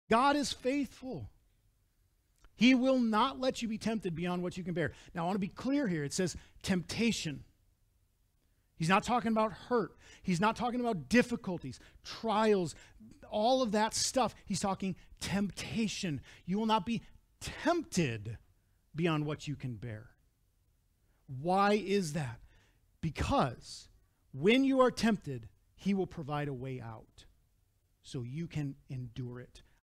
7.9.23 A Working Faith - Week 4
When we are dealing with temptation, it's important to remember that God is faithful, and we will not be tempted beyond what we can bear.  He will provide a way out so that we can endure it.  (1 Corinthians 10:13)